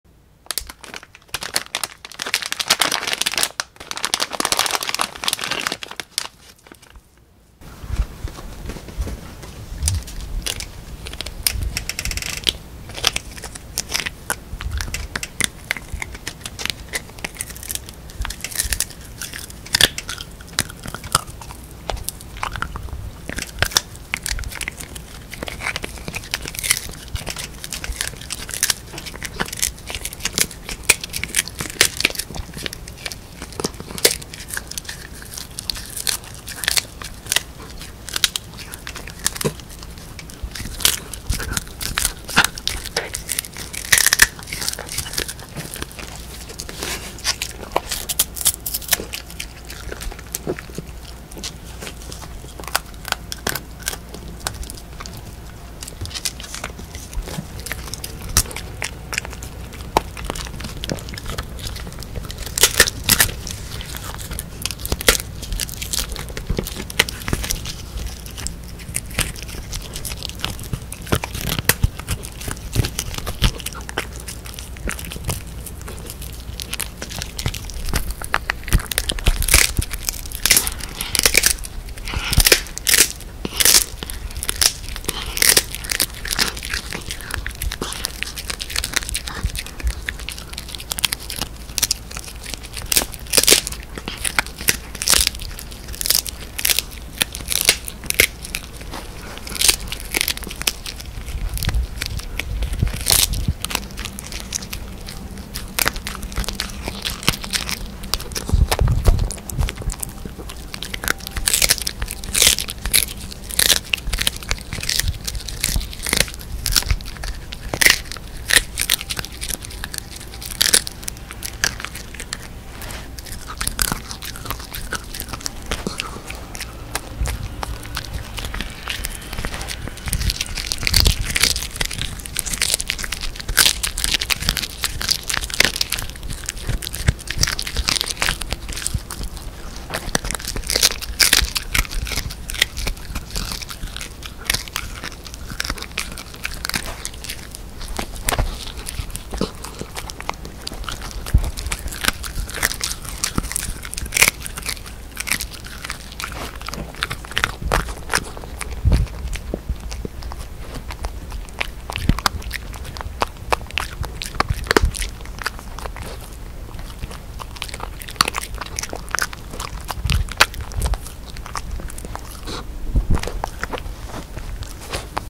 【ASMR 犬の咀嚼音】骨付き肉スペアリブを骨ごとバリバリ食べる犬はんぞう【ミニチュアダックスフンド】【音フェチ】
うたまる⇒ペキチワ(ペキニーズとチワワのmix)♀10か月。愛称"師匠"。

ASMR(音を楽しむ動画)に初挑戦してみました！今回はんぞうが夢中で食べたのは「鹿の匠」シリーズのスペアリブ(骨付き肉)です。しっぽをフリフリ♪おいしそうに食べてくれました＾＾

★ママのお気に入り動画

★パパのお気に入り動画

他